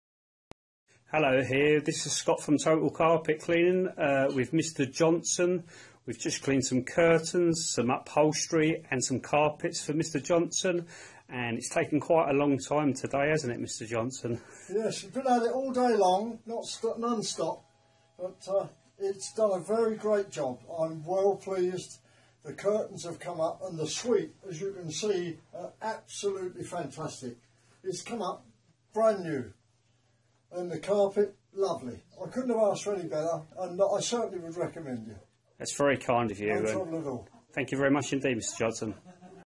Total Carpet Cleaning Testimonials
Testimonial after cleaning a suite of furniture and carpets